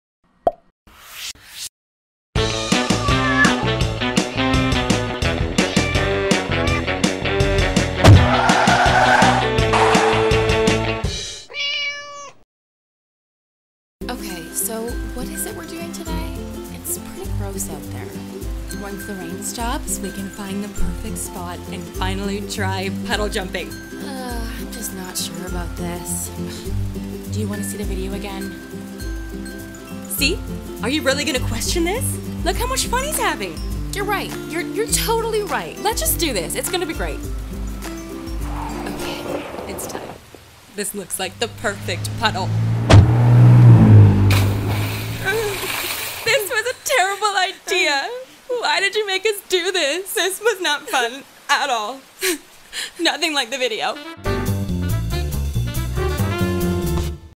The CatCar: Episode 8- "Puddle Jumping"
KarKat presents: "The CatCar" starring Kat Strain as "Fovea" and Karissa Strain as "Freya". The series follows the ventures of two sassy cats dealing with life's daily conundrums.

Written & Directed by Kat & Karissa Strain
Produced by Kat & Karissa Strain, Nicole Maroon
Edited by Alex Tomaszewski